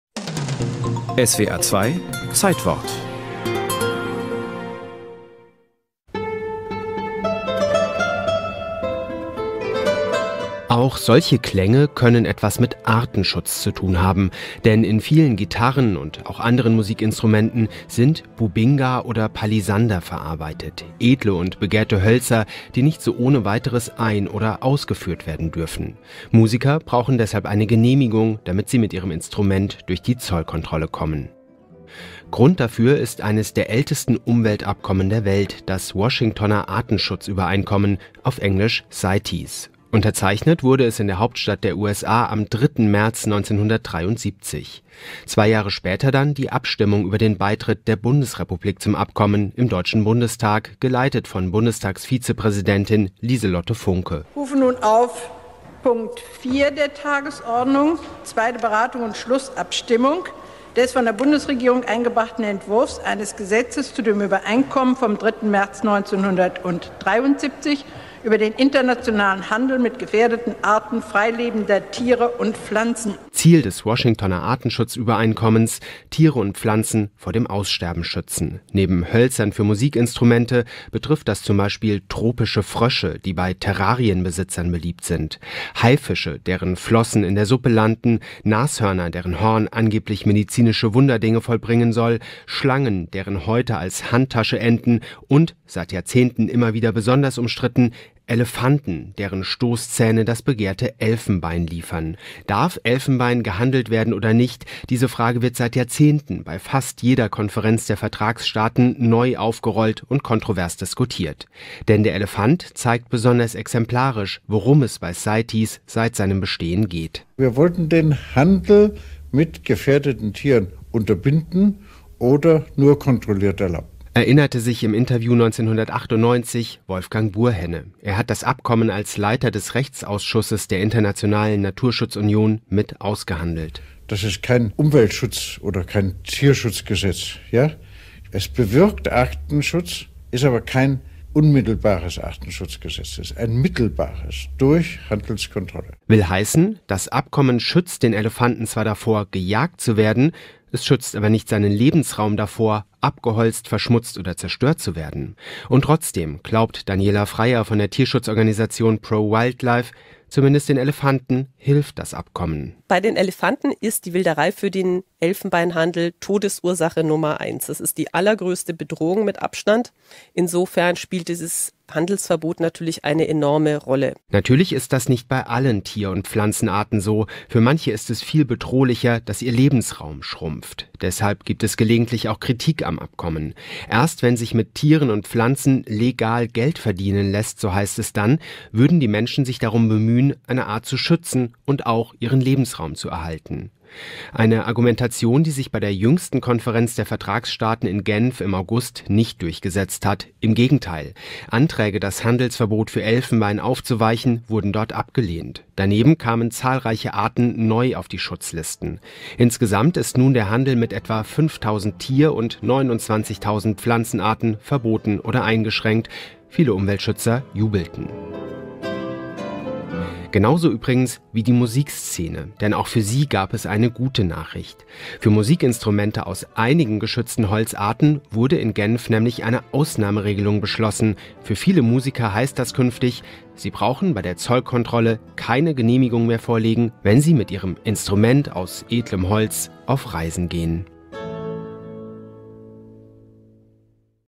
SWR 3.3.1973: Der Handel mit seltenen Tier- und Pflanzenarten wird verboten
183 Länder machen mit beim Washingtoner Artenschutzabkommen. Mit Handelsverboten für 5000 Tier- und 29.000 Pflanzenarten soll gefährdete Natur geschützt werden.